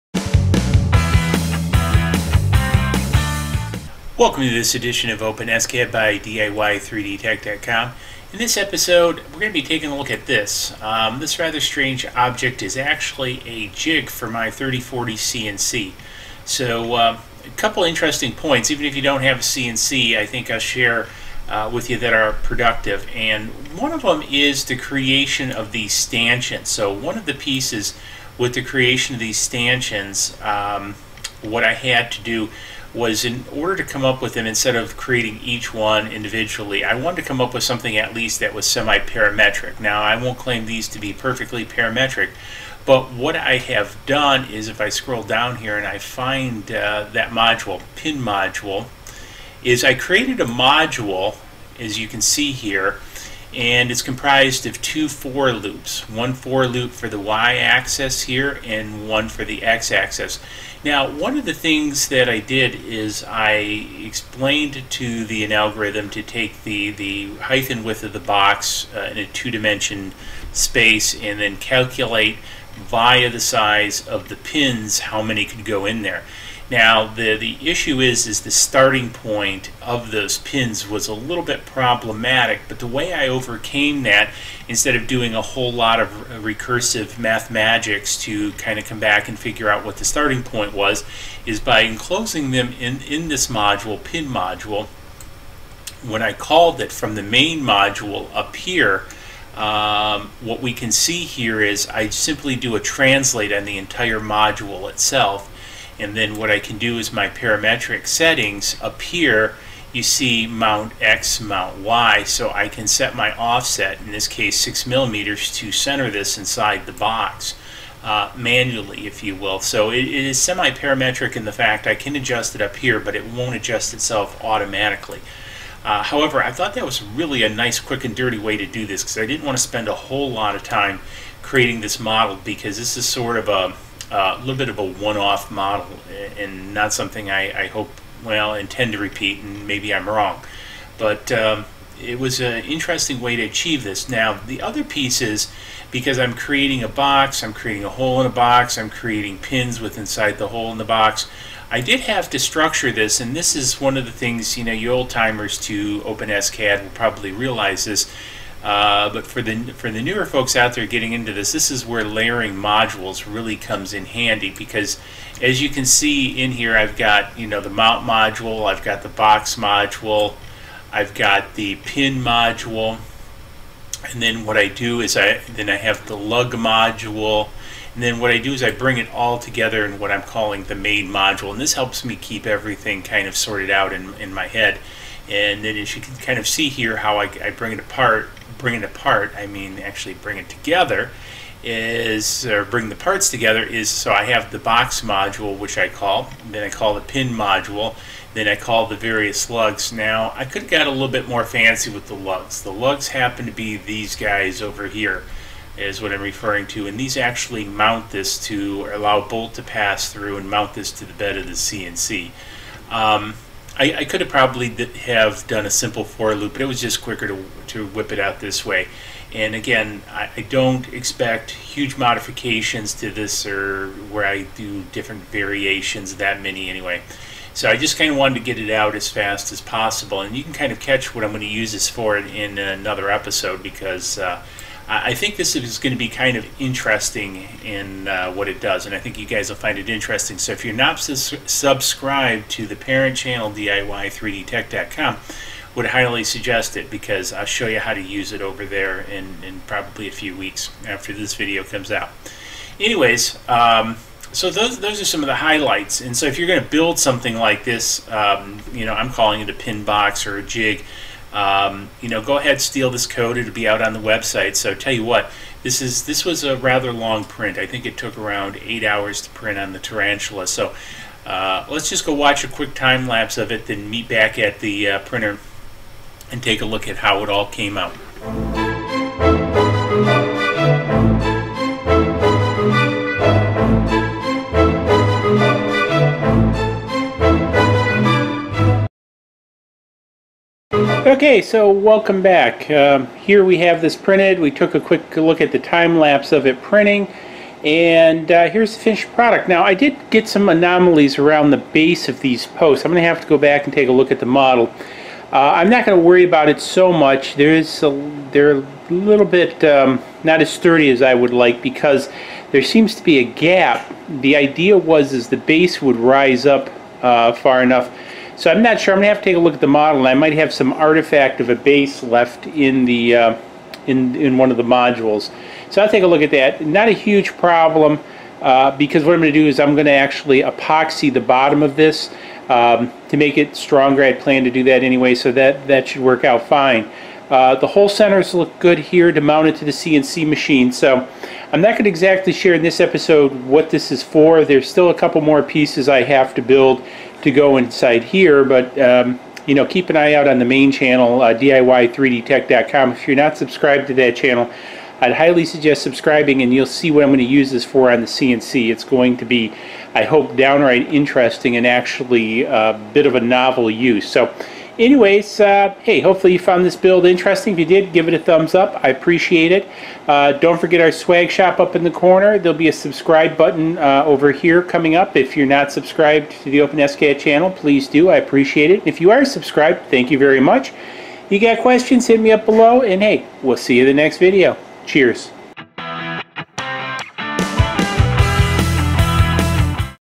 Open SCAD - CNC Jig 3D Printed!
for more information on this and many other projects! As in this episode we will be designing and then 3D print a rather interesting "jig" for the CNC machine.  There are a lot of handy tips and trips in this video to help you design your own projects!

For more information as well as the complete source code, please